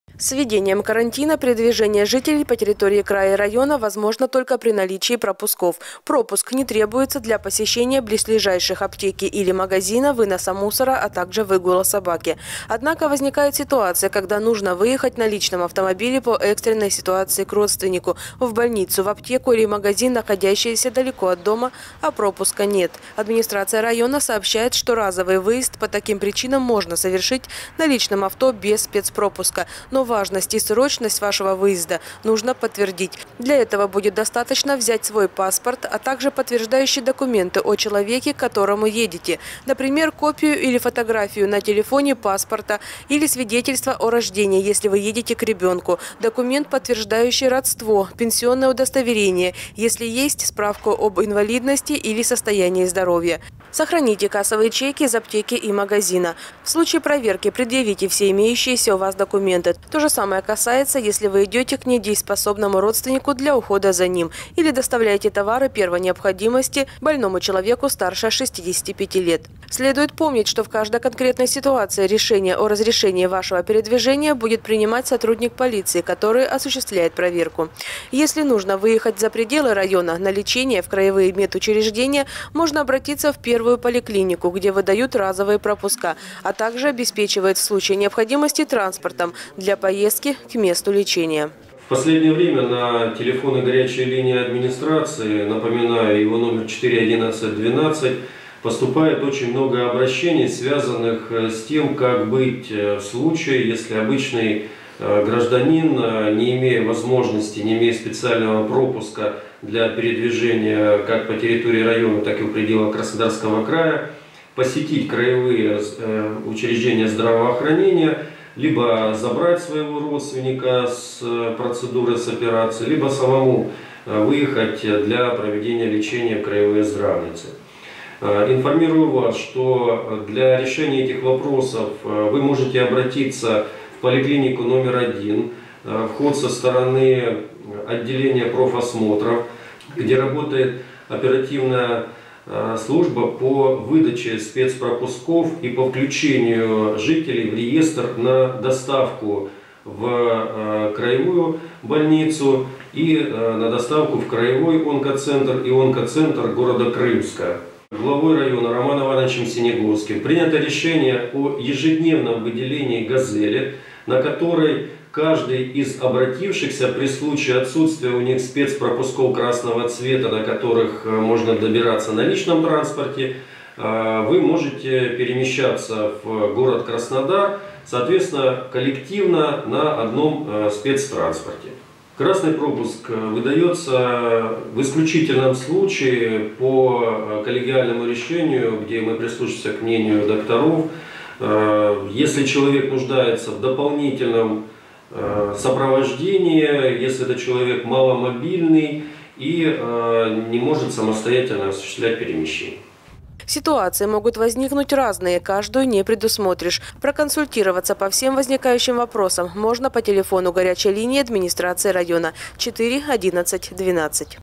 Разъяснение по пропускам
С введением карантина передвижение жителей по территории края и района возможно только при наличии пропусков. Однако, возникают ситуации,  когда  нужно экстренно выехать на личном автомобиле к родственнику, в больницу, в аптеку или магазин, находящиеся далеко от дома, а пропуска нет